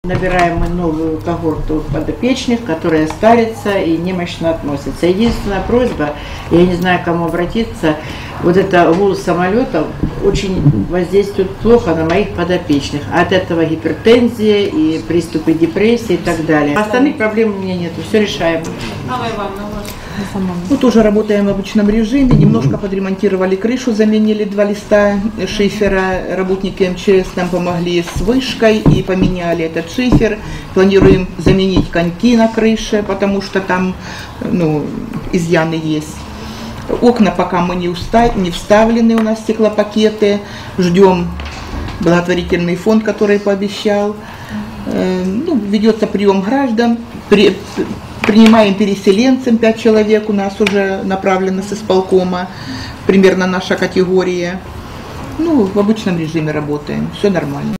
19 08 2014 Безпритульні
Буденні справи літніх людей та безхатченків під опікою територіального центру та будинку нічного перебування.